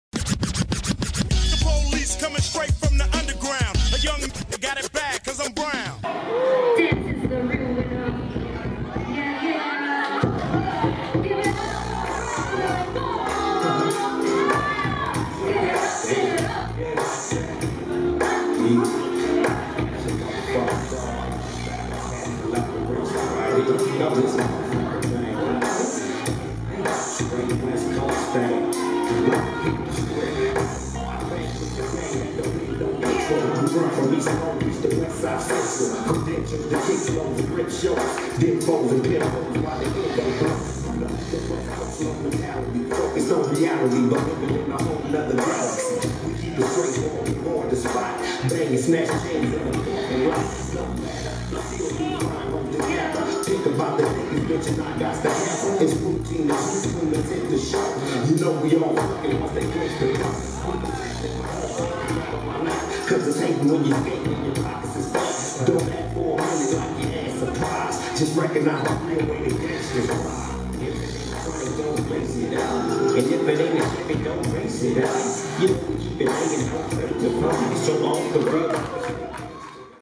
Romeo Pop | POPPING JUDGE SHOWCASE | SFTU VOL. 3
UTS Hiphop Society Presents: Straight From The Underground Vol. 3

POPPING JUDGE SHOWCASE
Romeo Pop

DJ: DJ Antho